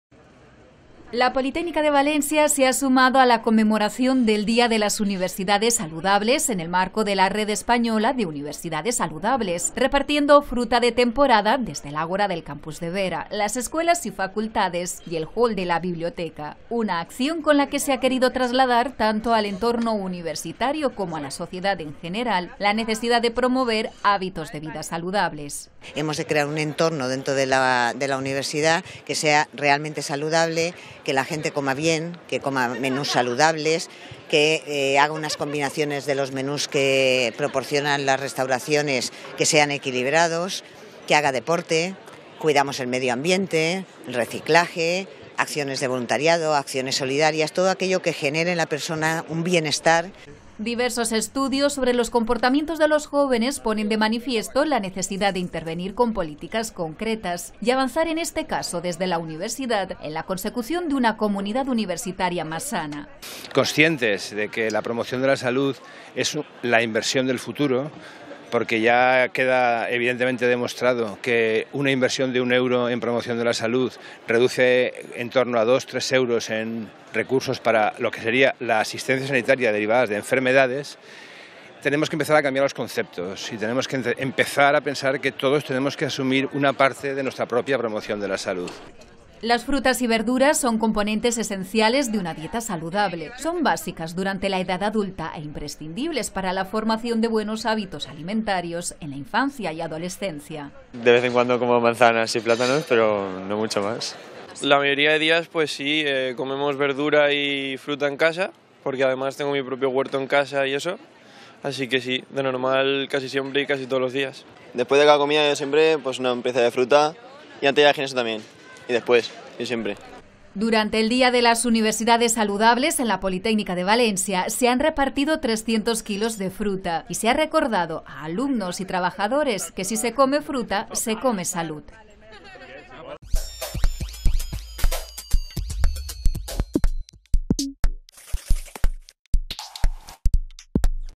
Noticia @UPVTV: Conmemoración del Día de las Universidades Saludables, [07-10-2015]
La Politècnica de València se ha sumado a la conmemoración del Día de las Universidades Saludables, en el marco de la Red Española de Universidades Saludables, repartiendo fruta de temporada desde el ágora del Campus de Vera, las escuelas y facultades y el hall de la Biblioteca. Una acción con la que se ha querido trasladar, tanto al entorno universitario como a la sociedad en general, la necesidad de promover hábitos de vida saludables.

Diversos estudios sobre los hábitos y comportamientos de los jóvenes ponen de manifiesto la necesidad de intervenir con políticas concretas y avanzar; en este caso, desde la Universidad, en la consecución de una comunidad universitaria más sana.

Las frutas y verduras son componentes esenciales de una dieta saludable. Son básicas durante la edad adulta e imprescindibles para la formación de buenos hábitos alimentarios en la infancia y adolescencia.

Durante el Día de las Universidades Saludables, en la Politècnica de València se han repartido 300 kilos de fruta y se ha recordado a alumnos y trabajadores que si se come fruta… se come salud.